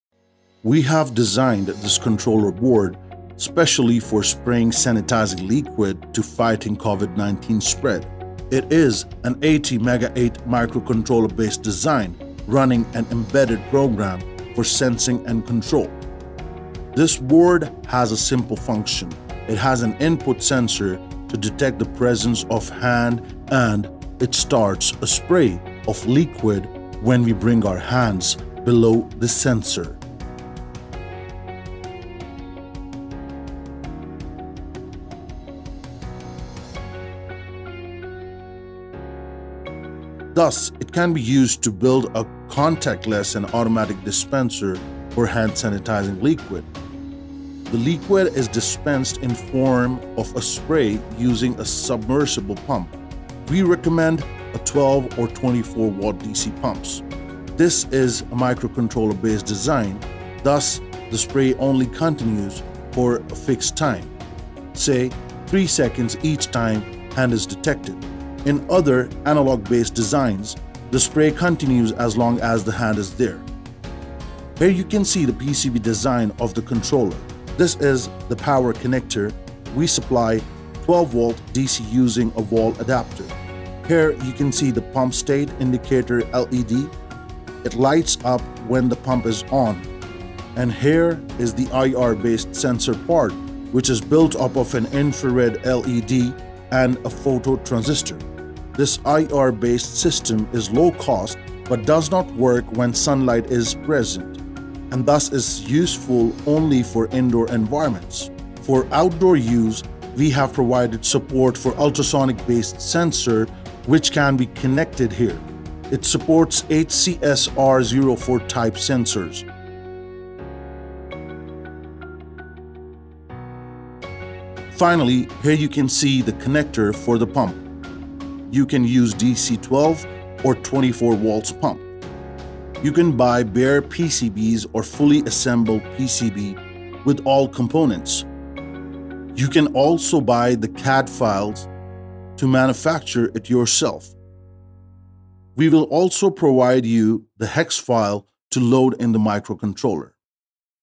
Automatic Hand Sanitizer Dispenser Control Card ATmega8 Based - Fight COVID-19
This simple microcontroller based system to sense hands and automatically spray sanitizing liquid. It works with both infrared and ultrasonic based sensors. Built using a low cost and easily available ATmega8 MCU